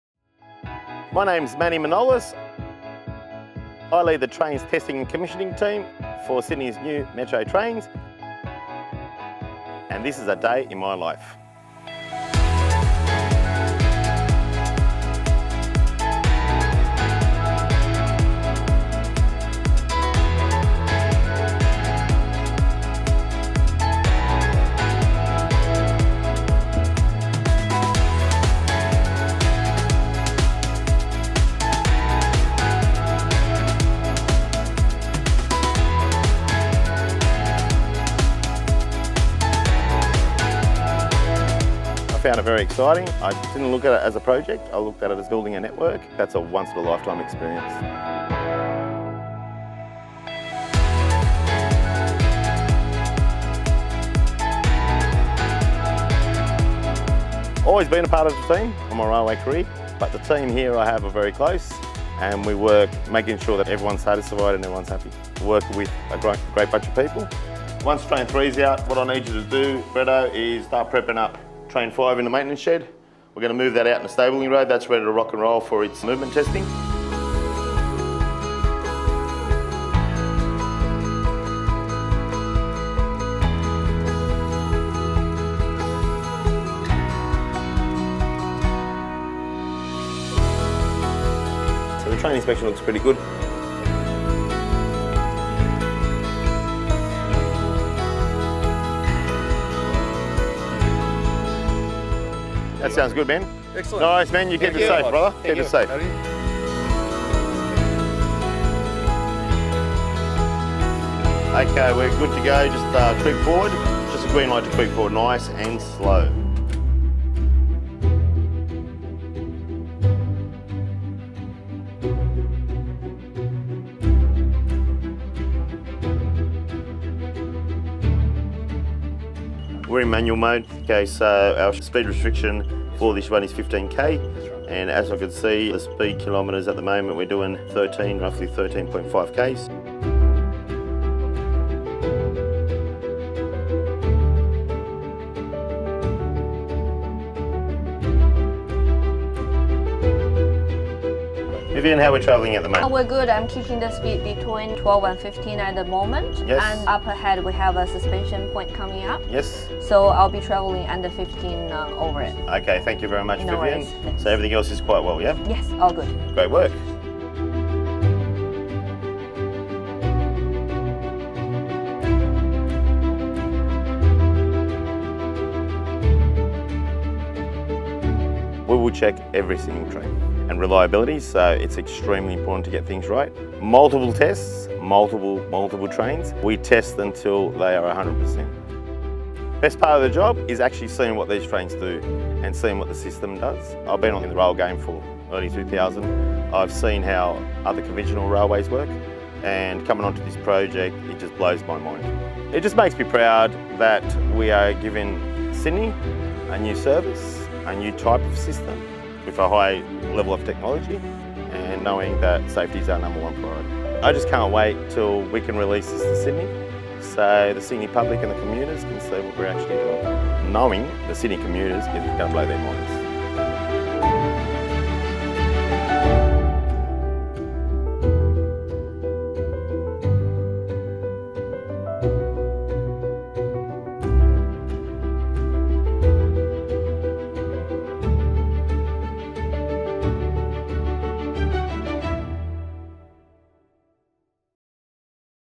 Sydney Metro: Manny Manolis, a day in the life
Meet Manny Manolis, the head of the Sydney Metro train testing and commissioning team.

Australia’s biggest public transport project – and the nation’s most advanced commuter railway – uses fully-automated driverless trains.

Manny and his team from Sydney Metro builder Northwest Rapid Transit test every part of the trains during the commissioning process, travelling thousands of kilometres.

Join Manny for a day in his working life.